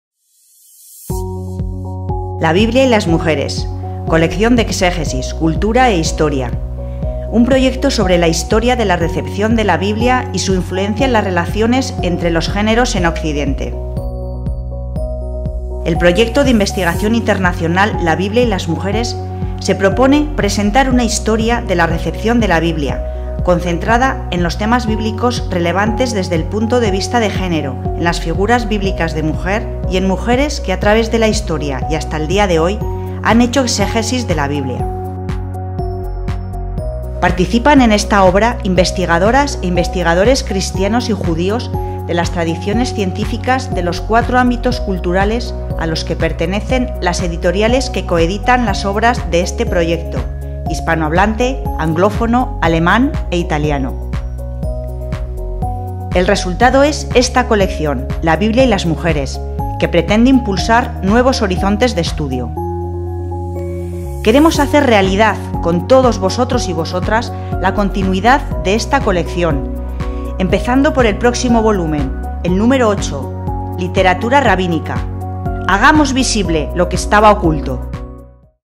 La Biblia y las Mujeres
Colección sobre la historia de la recepción de la Biblia concentrada en los temas bíblicos relevantes desde una perspectiva crítica de género, en las figuras bíblicas de mujer, y en mujeres que a través de la historia y hasta el día de hoy han hecho exégesis de la Biblia. Proyecto internacional que se publica simultáneamente en español, alemán, inglés e italiano.